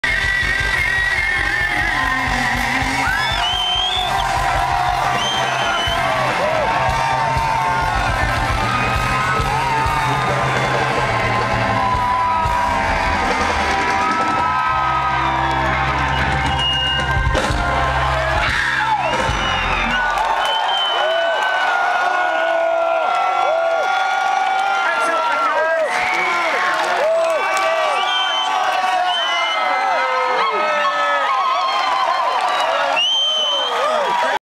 Mike Patton decides to film the audience at Ilosaarirock 2010, part II
Juuust missed high-fiving him.